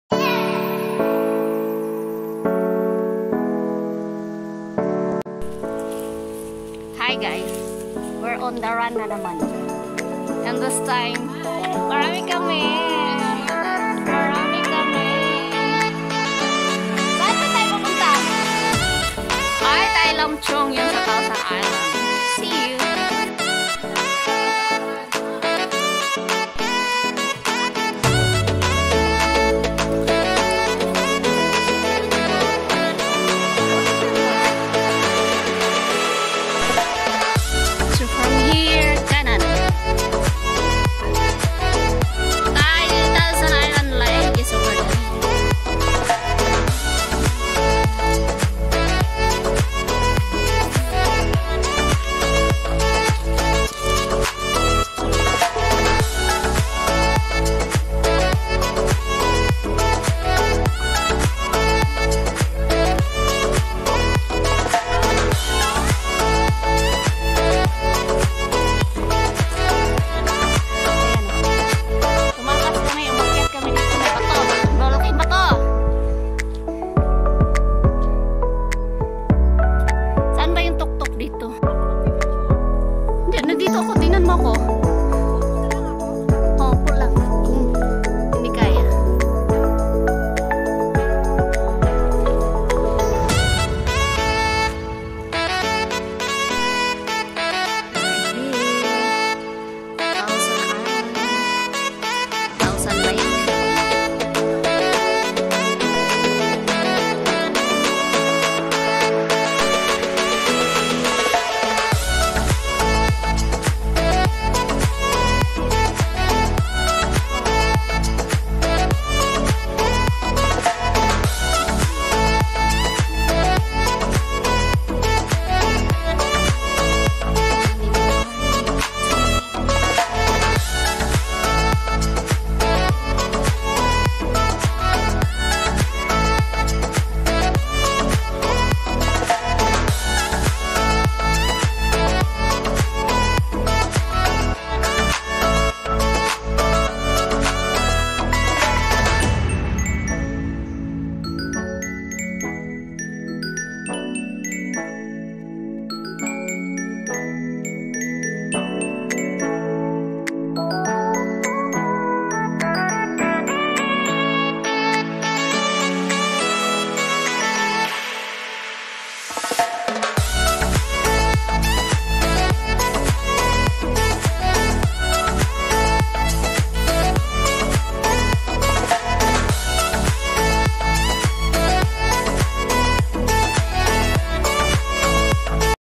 How To Hike Tai Lam Chung Reservoir - Thousand Islands Lake hongkong #2022
HK PINAY HIKER OFW
FAMILY HIKE
Friendly Hike

How to get there:
Get to Tuen Mun MTR station Exit F2.
Turn right across the road then turn right again, you will see the market head to the front of the market.
Take green minibus 43 in front of the San Hui Market on Ho Pong Street.
Get off at the end in So Kwun Wat Tsuen and its 20 minutes ride.
Return via green minibus 43 to Tuen Mun MTR station. Enjoy.